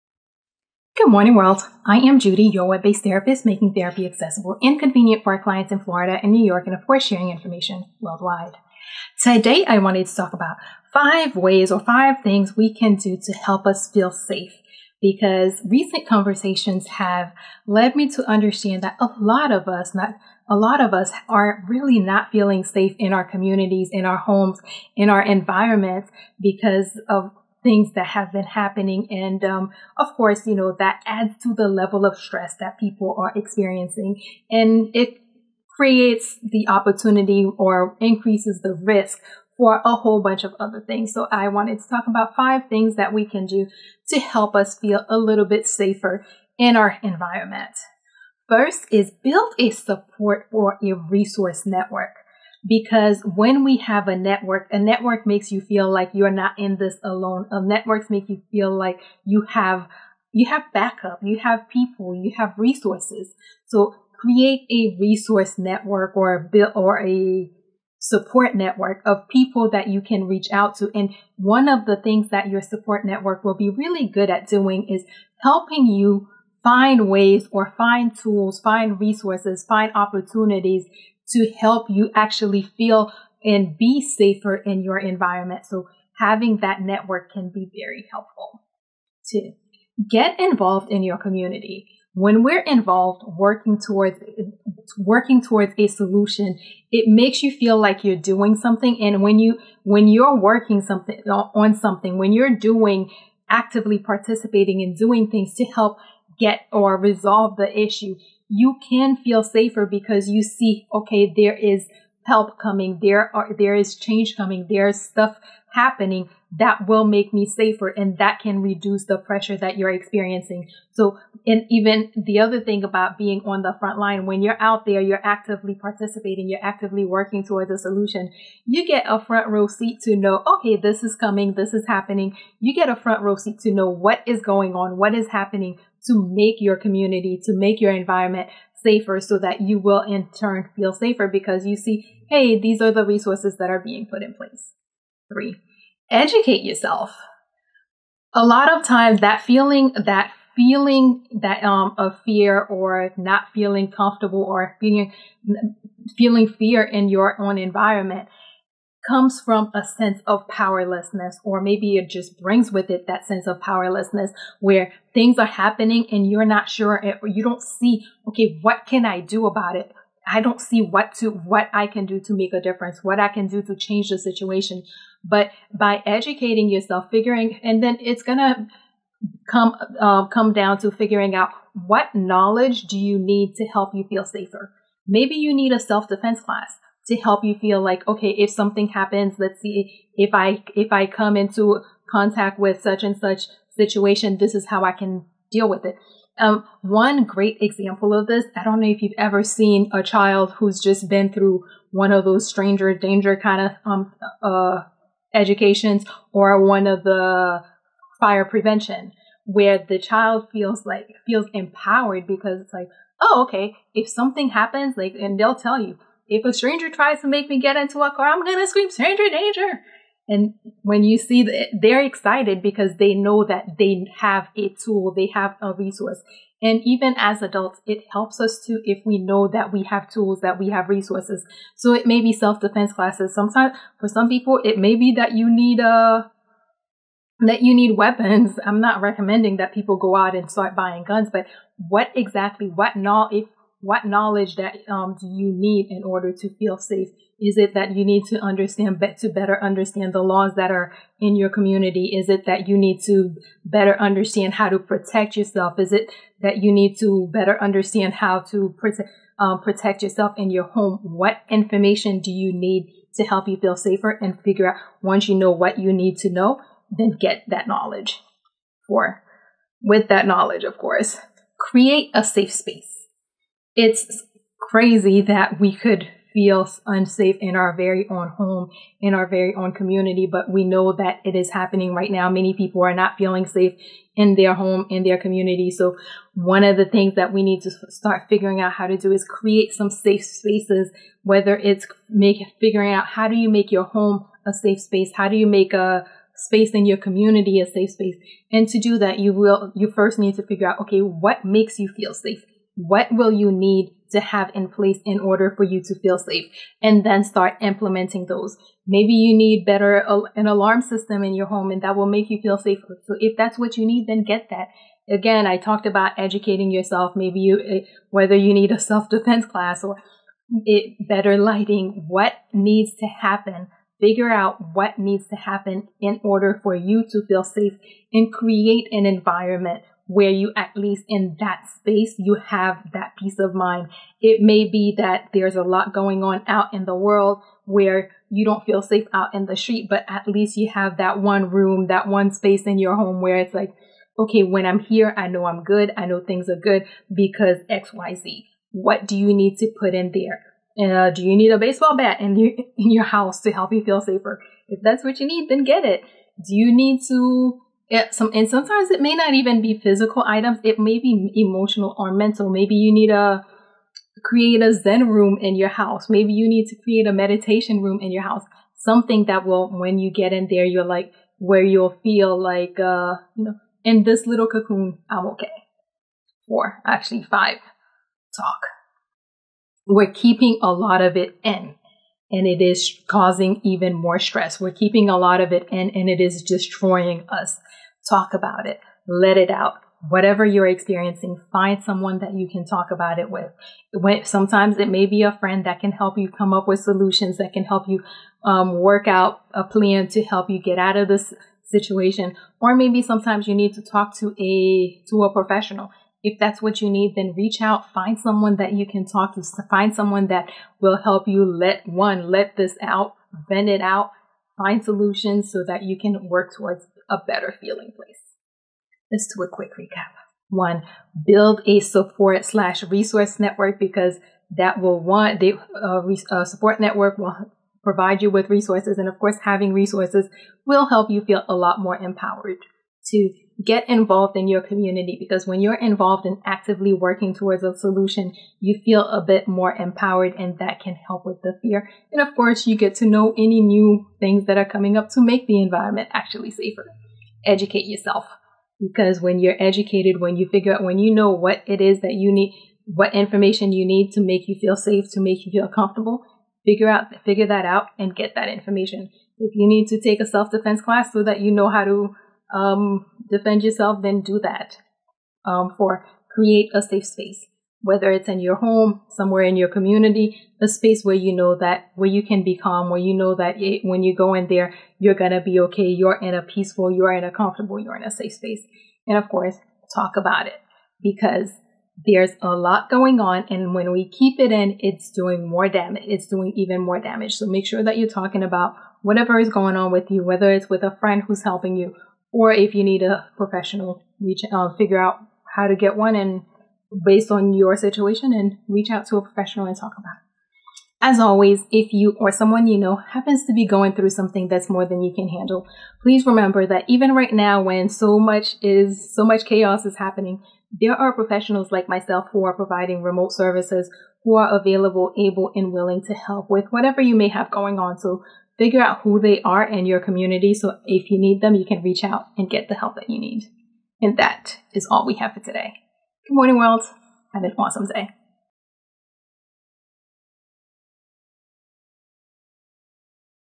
Five (5) Tips To Help You Feel Safe When World Feels Unsafe (9)
In light of recent event, many people have reported that they are not feeling safe in their community and sometimes even in their own home. Even worse is that sense of powerlessness to do anything about it. In a society of racial injustice where Black people feel under attack and the law not only seems to not protect them but it also punishes those who dare to protect themselves, many people feel they are in an impossible position. It is a sad state when you have to remind your community that Black Lives Matter only to be attacked for it. Demanding that they Let Us Live and getting push back for doing so the wrong way. Law enforcement is failing Black People. The Justice system is failing Black People. The American Society and its culture of systemic racism is failing Black people. But we still must find and create our own sense f safety in this world that feels so unsafe.